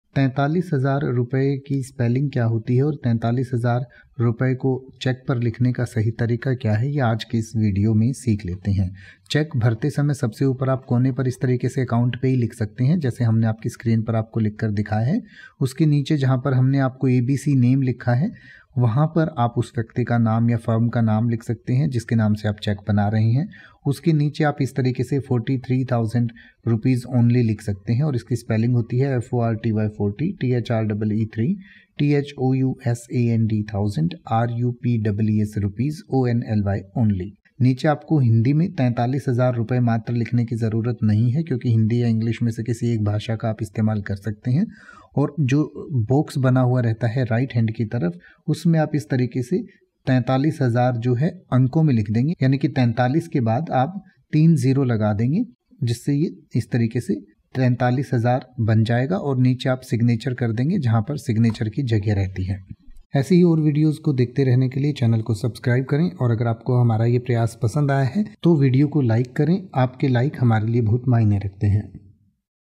43000 ki spelling | How to write 43000 on Bank cheque | 43000 in words | 43000 ki English
43000 ki spelling | How to write 43000 on Bank cheque | 43000 in words | 43000 ki English

तो बस एक मिनट में सीखिए 43000 को
Let’s learn Hindi Meaning of 43000 in detail.

43000 का अंग्रेजी में अर्थ होता है
43000 रुपये को Cheque पर लिखने का सही तरीका :

 Forty three thousand Rupees only
 तैंतालीस हज़ार रुपये मात्र

43000/-

account payee